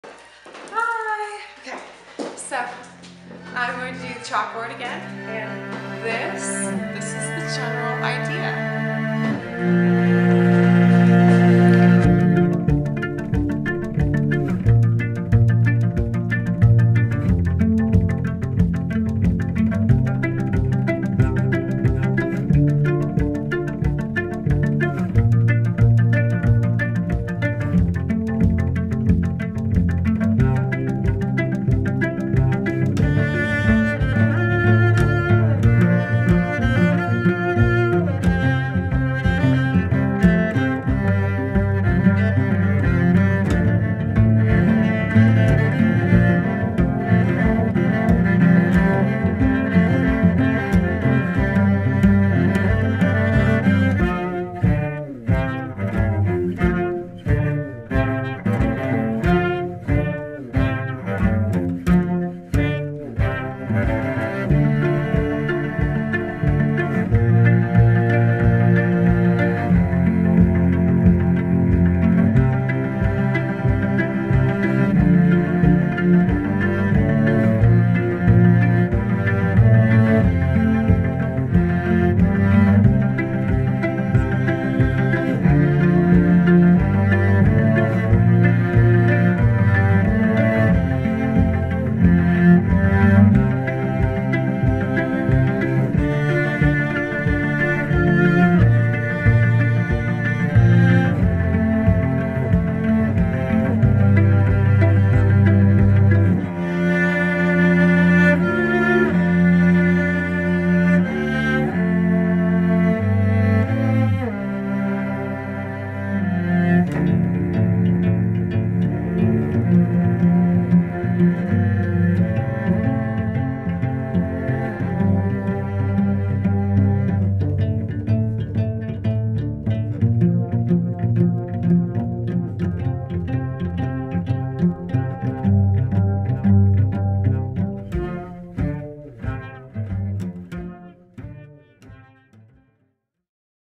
Time lapse: Orchid & Ram Skull in chalk
Quick chalk board drawing in honor of Georgia O'Keefe's birthday!